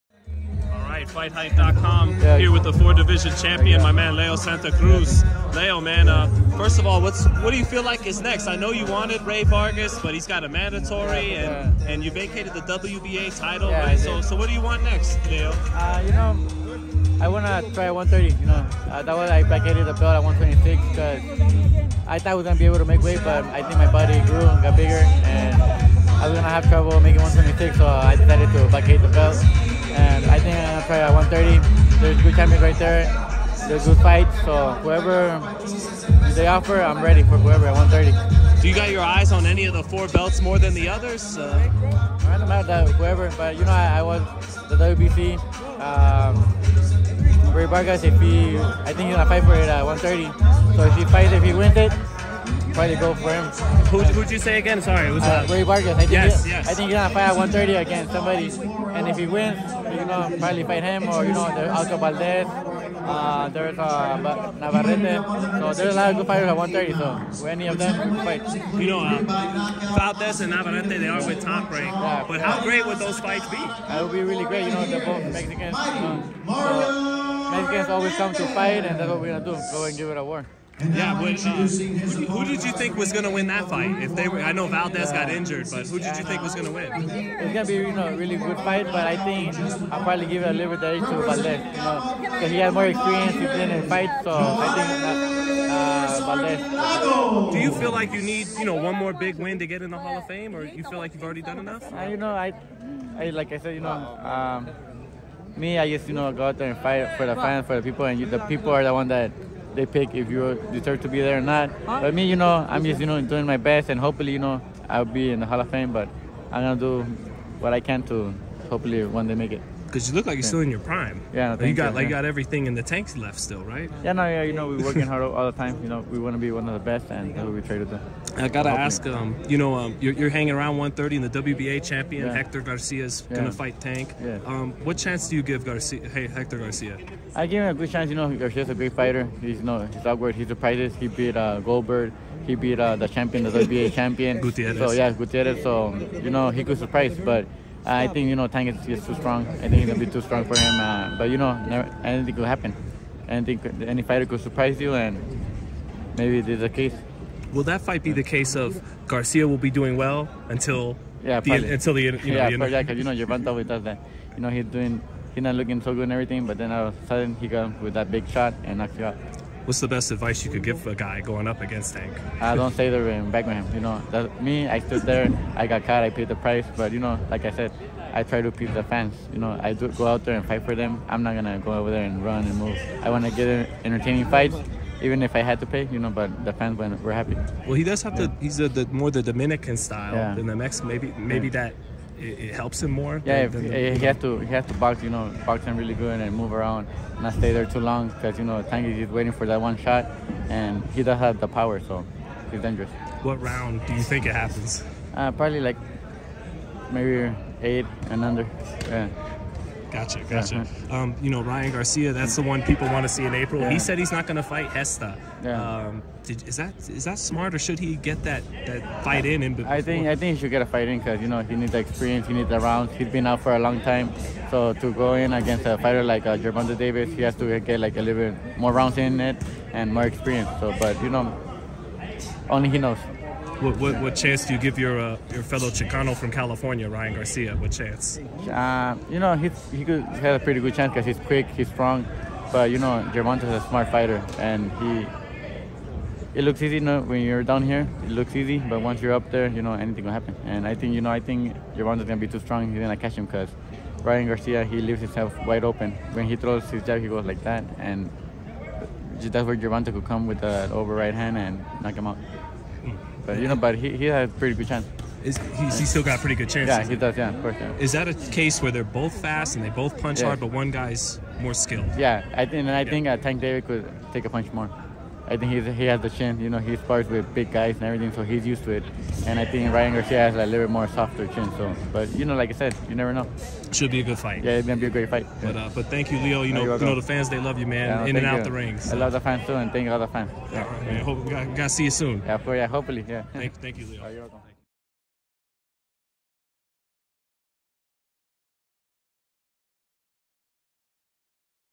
LEO SANTA CRUZ TELLS HOW GERVONTA DAVIS WILL KNOCK OUT RYAN GARIA; TRUTH ON "SOFTER CHIN" MISTAKE
Top undefeated lightweight contenders Michel Rivera and Frank Martin are moments away from their anticipated December 17 showdown at the Cosmopolitan in Las Vegas, Nevada. You don't want to miss what went down before, during, and after the fight. Check it out!